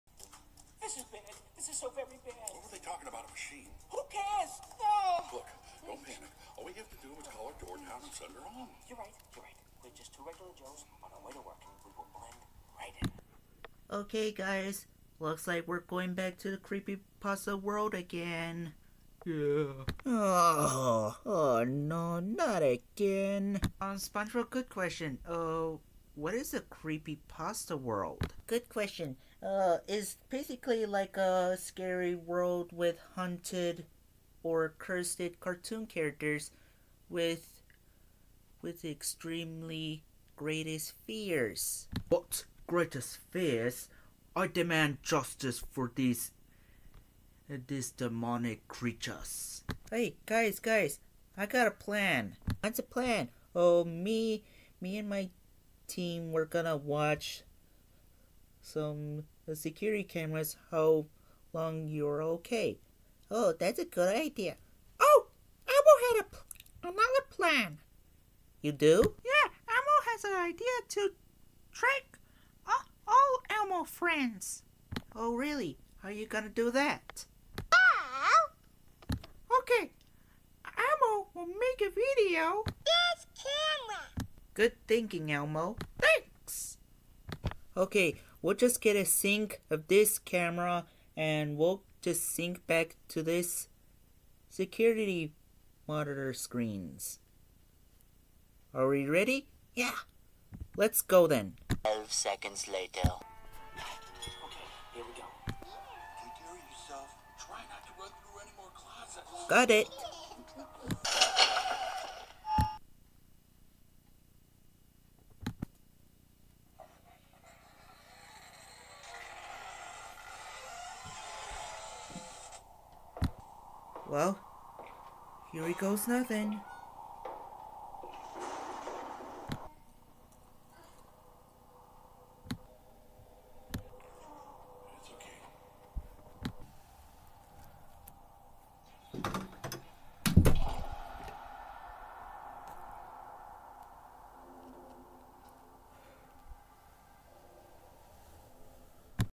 TFS Team VS Mane 6.EXE (Round 2) Part 1
SpongeBob and his Team decided to go back to the Creepypasta World to find the truth, finding some unknown items and challenging Creepypasta characters.